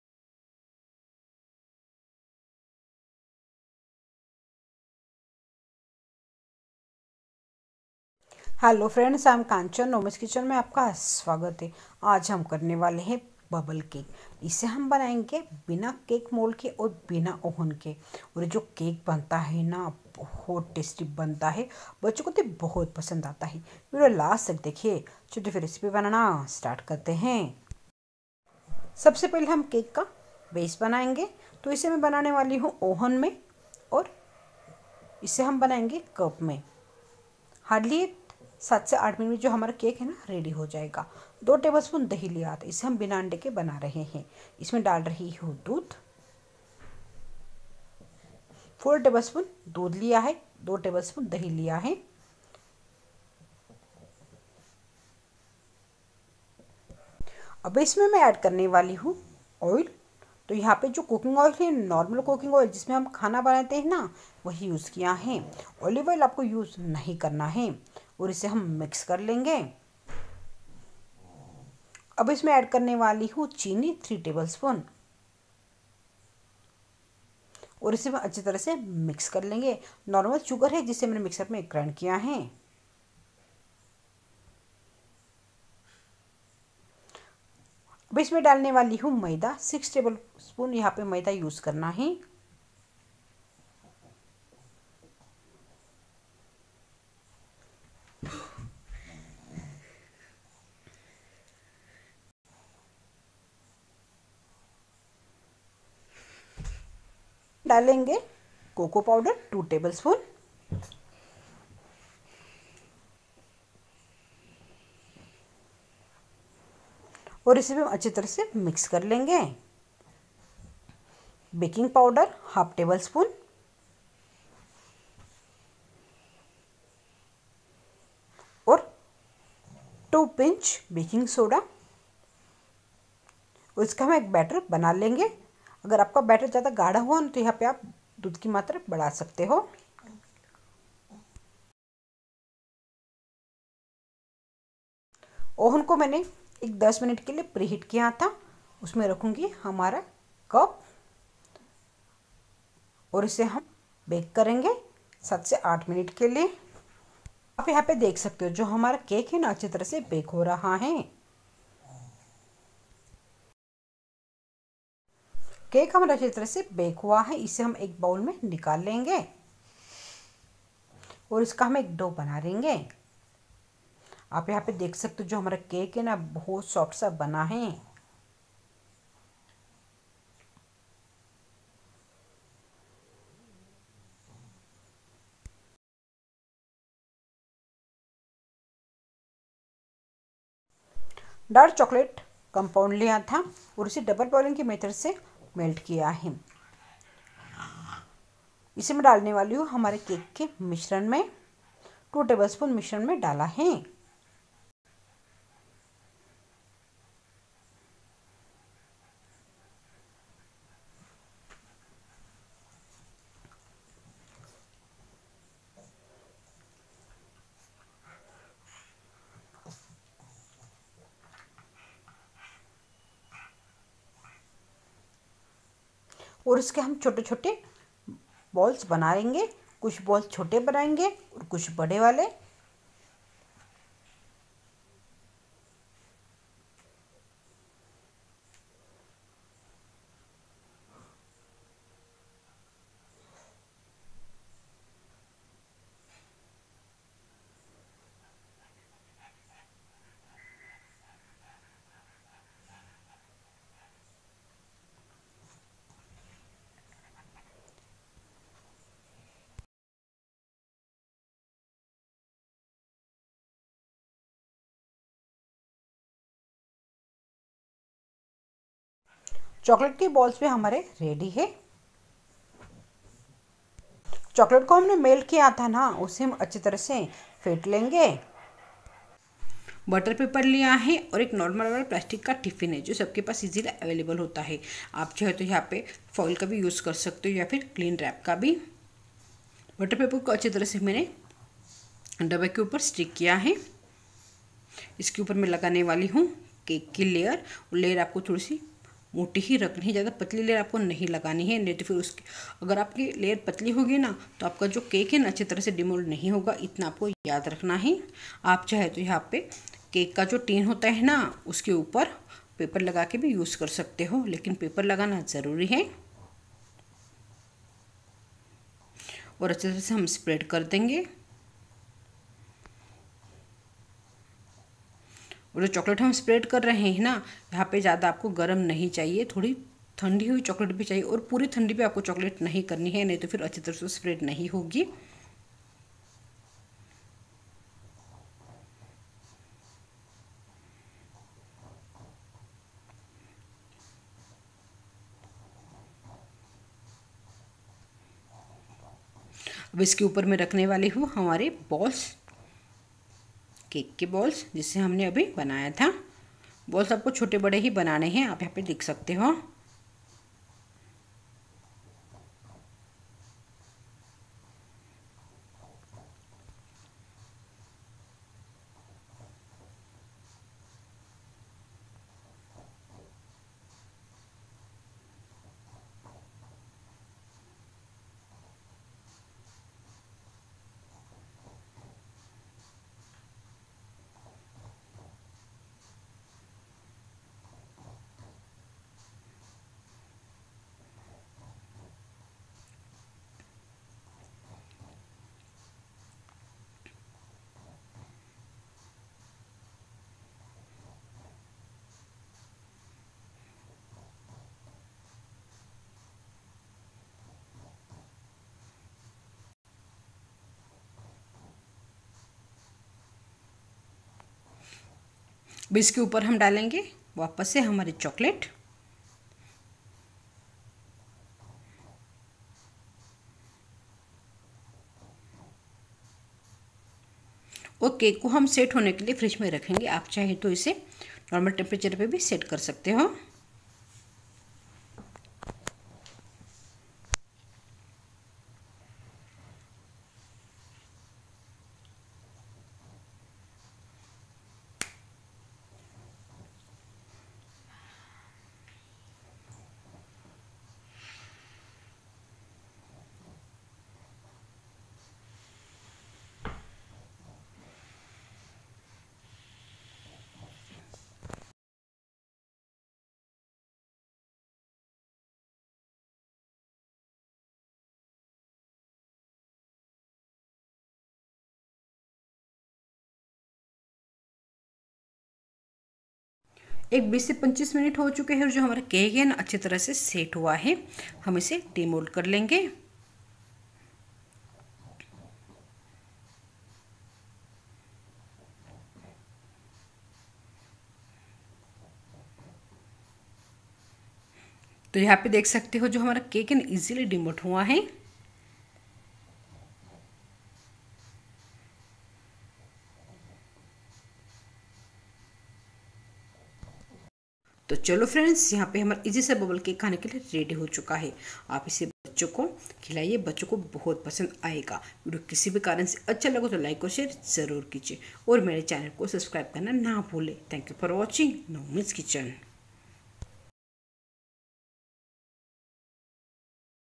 Only 3 ingredients Bubble Cake in 15 Minutes Without Eggs/Butter/Mould | चॉकलेट बबल केक बिना मौल्ड
dark chocolate slab
cake